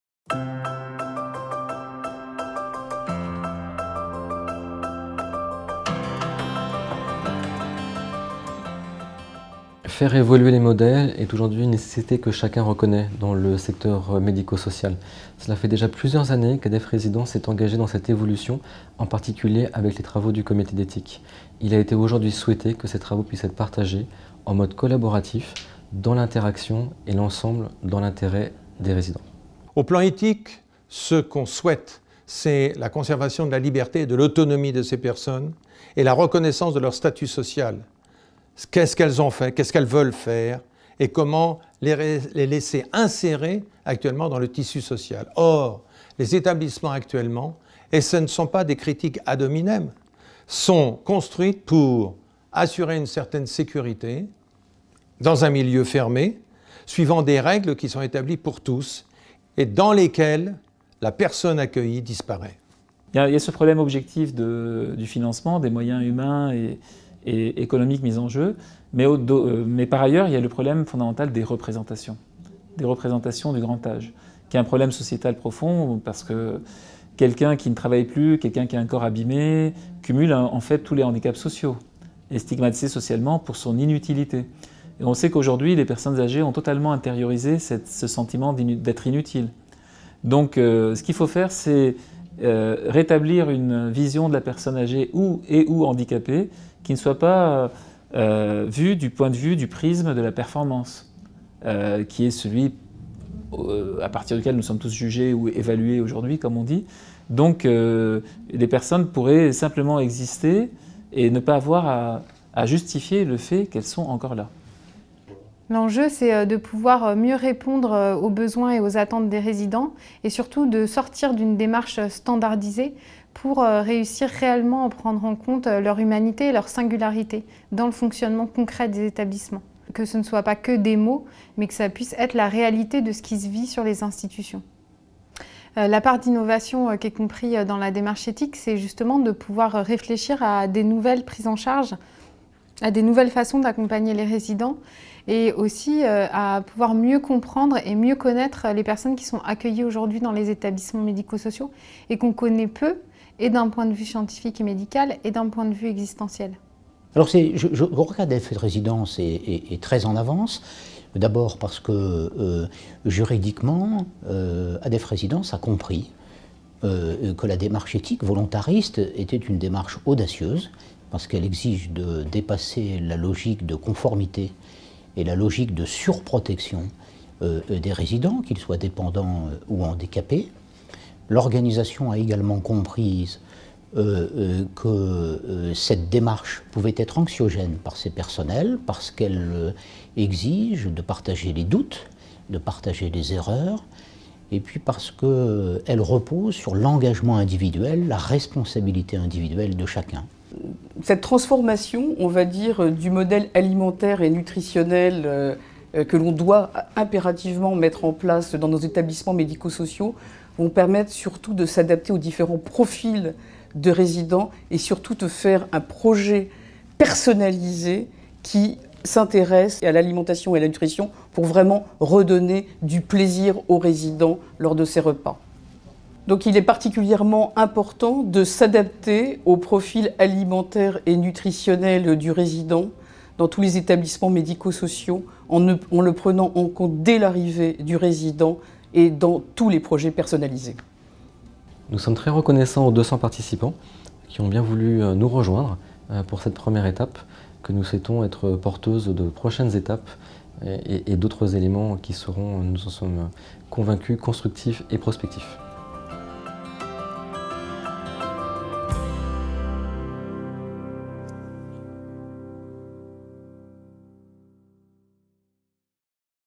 Colloque CEAR : l’éthique au service d’une nouvelle conception des Etablissements Medico-Sociaux
Interview d'Yves Claisse, avocat associé du Cabinet Claisse & Associés - Arnaud Gallet, directeur de la communication Adef Résidences - Alain Grimfeld, Médecin – Yannis Constantinidès, philosophe – Sabrina Blot-Leroy, Directrice des Sciences Humaines et Sociales d’Adef Résidences – Laure Cloarec-Blanchard, Médecin Gériatre, Adef Résidences, au colloque du 4 décembre 2018, organisé par le Comité d’Éthique Adef Résidences.